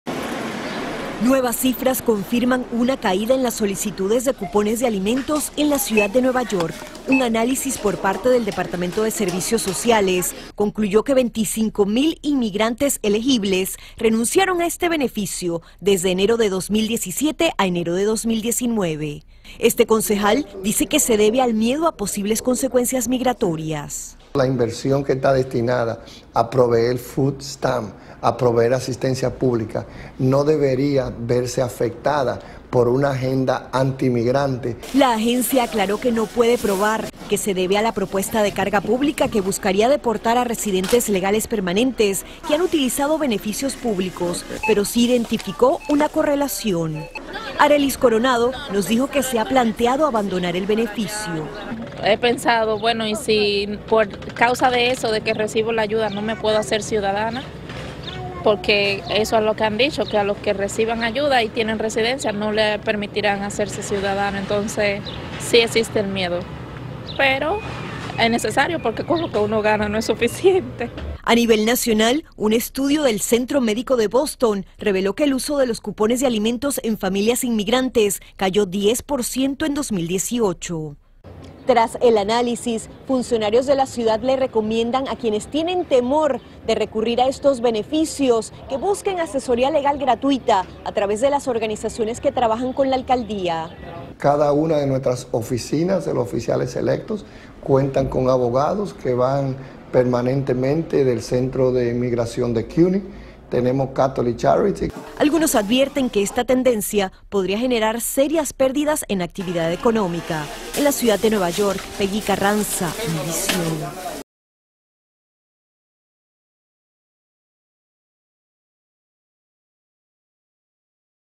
Miles de inmigrantes en Nueva York renuncian a cupones de alimentos por miedo de perder su estatus
Un análisis por parte del Departamento de Servicios Sociales concluyó que 25,000 inmigrantes elegibles para el Programa Asistencial de Nutrición Suplementaria (SNAP) renunciaron a este beneficio desde enero de 2017 al mismo mes de 2019.